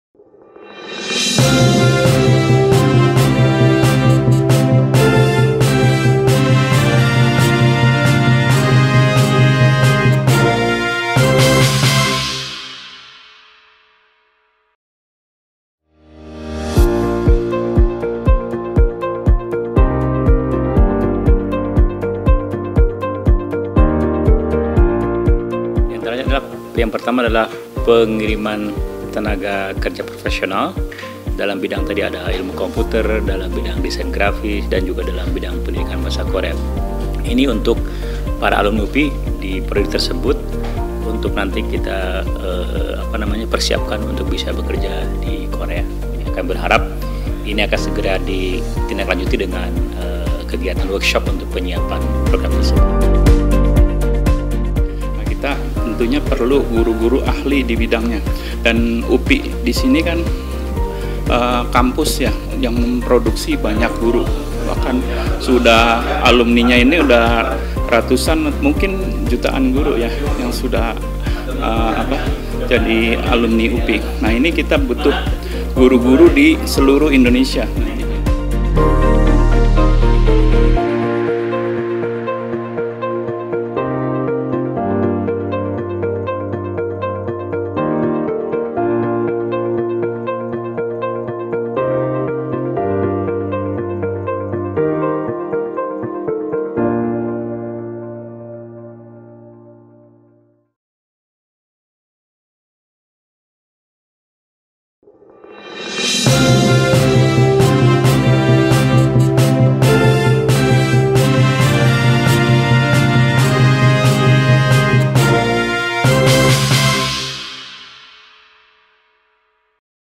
UPI dan PT. Tchain Digital Innovation menandatangani MoU kerja sama
UPI dan PT. Tchain Digital Innovation menandatangani MoU kerja sama  untuk meningkatkan pembelajaran via online (12/12).